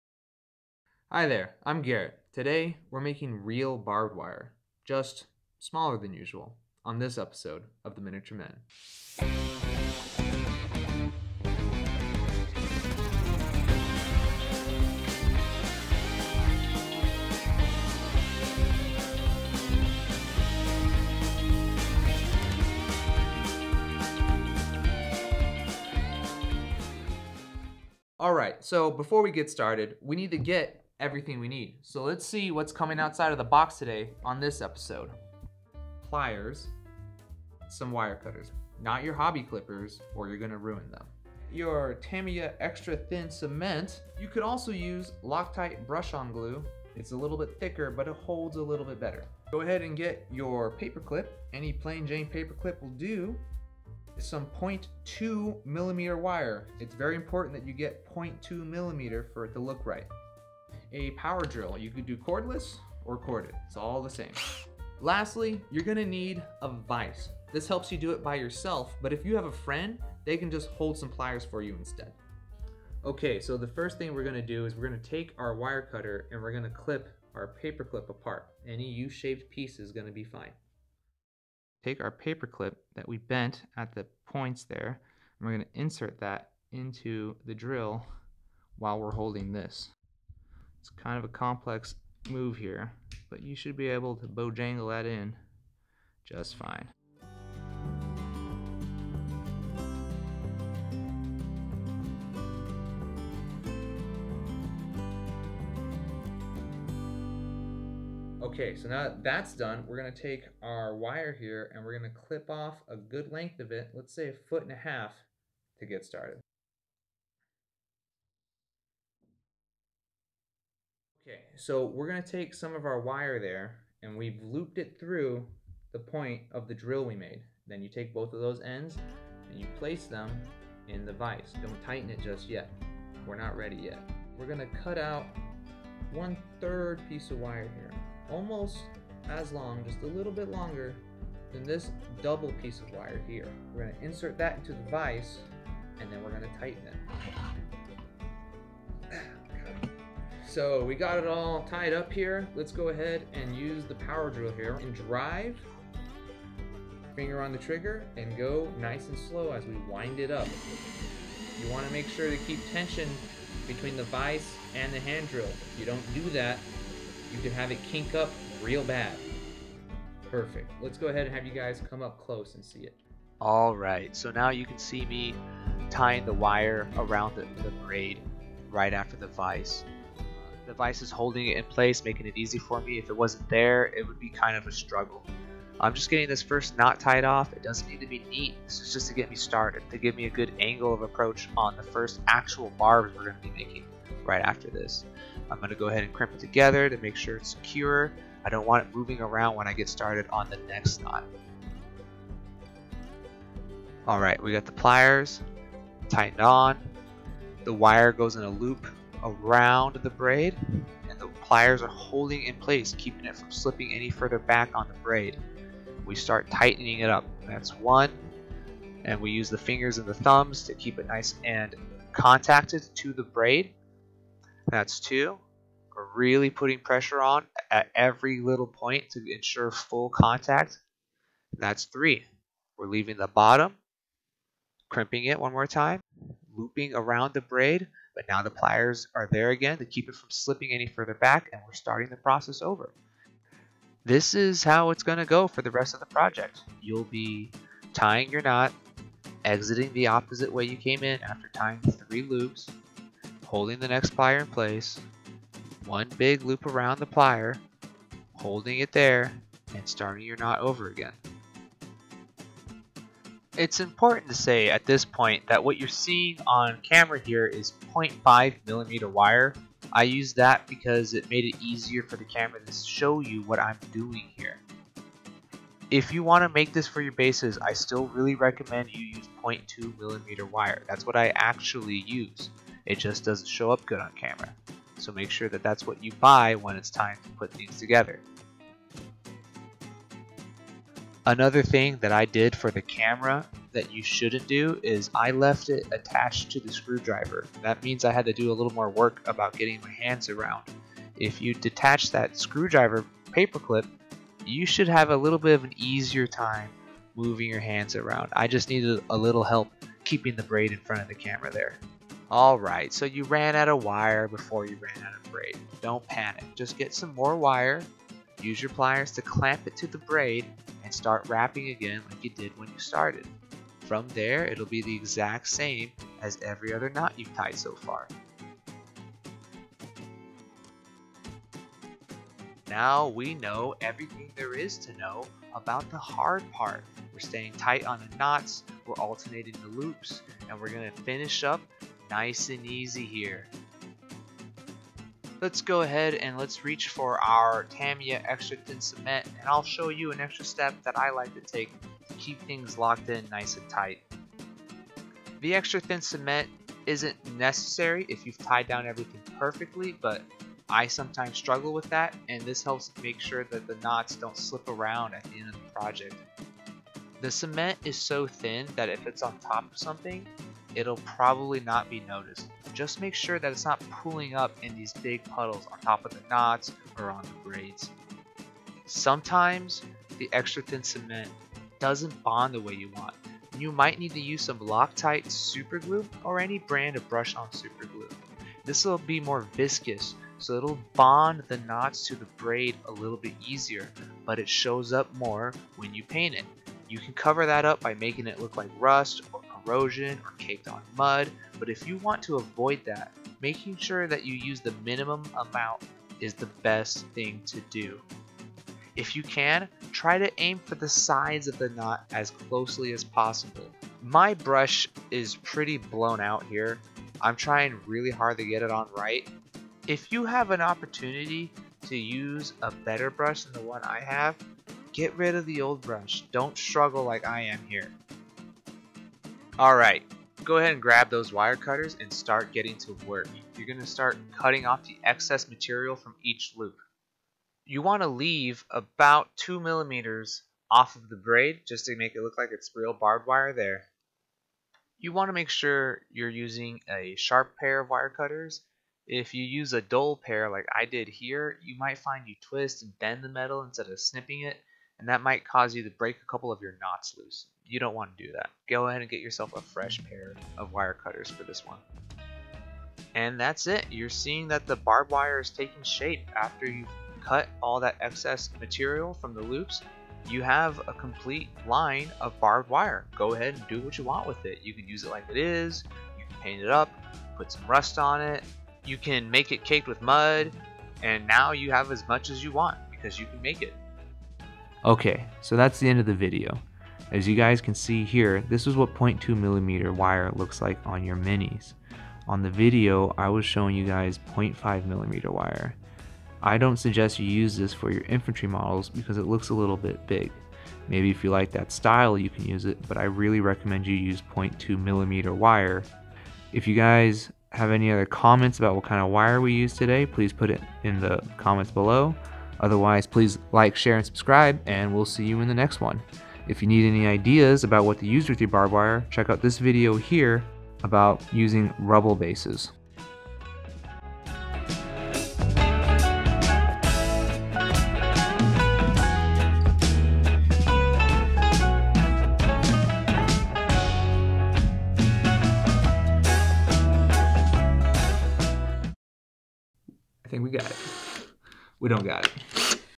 Miniature Barbed Wire
Today we show you how to make miniature barbed wire that works great for 40k or any other miniatures.

Materials
Tamiya Extra Thin Cement
Jumbo Paper Clips
0.2mm Wire
Wire Cutters
Bent Nose Plyers